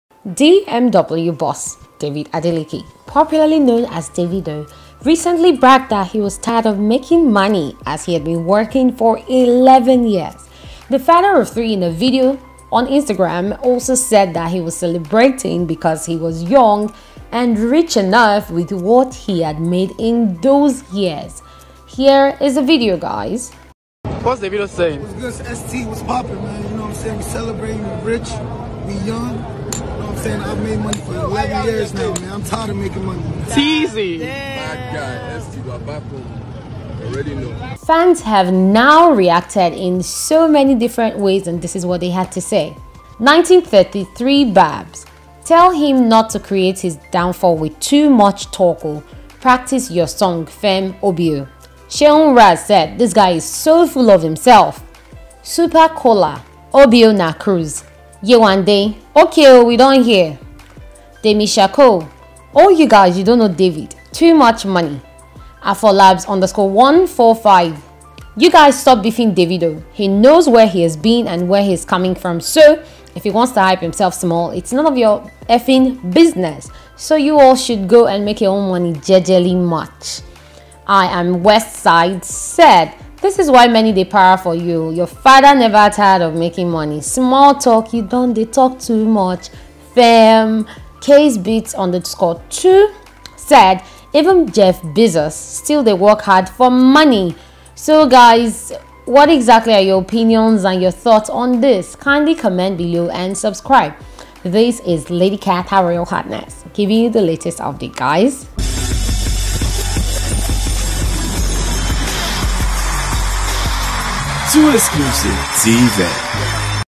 Davido Laments About His Wealth. Fans Respond
Nigerian singer David Adeleke, popularly known as Davido, recently bragged that he was tired of making money as he had been working for 11 years.